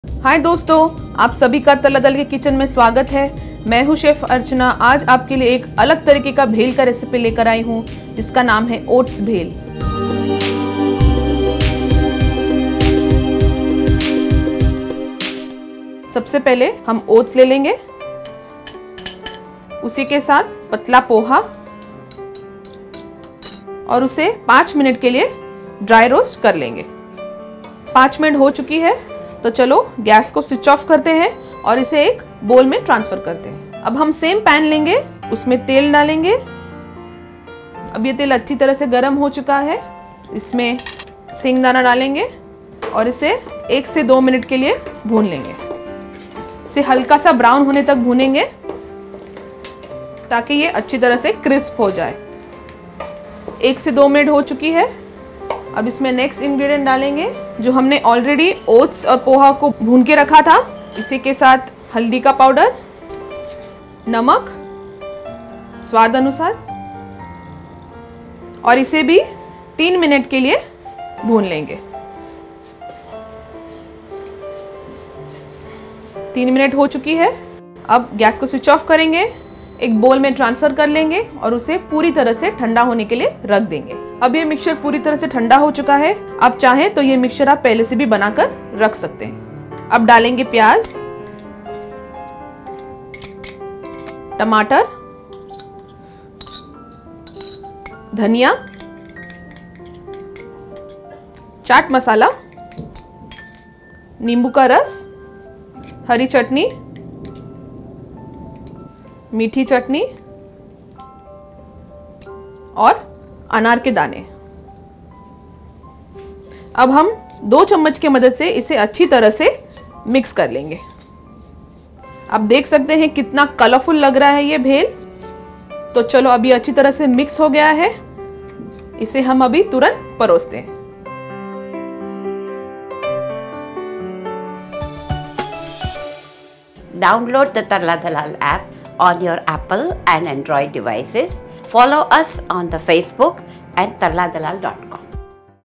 ओटस् भेल Oats Bhel, Healthy Snack for Kids by Tarla Dalal
Oats Bhel,

Oats Bhel, Healthy Snack for Kids

Who does not like to munch on bhel, the all-time favourite Indian roadside snack? The spectra of flavours and textures packed into a bowlful of bhel is truly mind-boggling and makes it a hot favourite with young and old alike. We recreate the same wonderful experience in a healthier way, for kids to enjoy as an after-school treat. Oats and poha are roasted till crisp and perked up with crunchy peanuts, tongue-tickling chutneys and juicy veggies. Do not forget to add a dash of lemon juice as it is instrumental in boosting the flavours of the Oats Bhel. You can also treat your kids to other healthy snacks like the Moong Sprouts Dosa and Multigrain Palak Paneer Roti.

Serves 3.

1½ cups quick cooking rolled oats
½ cup thin beaten rice (poha)
2 tbsp oil
2 tbsp raw peanuts
½ tsp turmeric powder (haldi)
Salt to taste
¼ cup finely chopped onions
¼ cup finely chopped tomatoes
2 tbsp finely chopped coriander (dhania)
1 tsp chaat masala
½ tsp lemon juice
1½ tbsp green chutney
1 tbsp sweet chutney
¼ cup pomegranate (anar)

1. Combine the oats and poha in a broad non-stick pan and dry roast on a medium flame for 5 minutes. Remove and keep aside.
2. Heat the oil in the same broad non-stick pan, add the peanuts and sauté on a medium flame for 1 to 2 minutes.
3. Add the oats-poha mixture, turmeric powder and salt and sauté on a medium flame for 3 minutes. Keep aside to cool completely.
4. Transfer the mixture into a deep bowl, add all the remaining ingredients and toss well.
Serve immediately.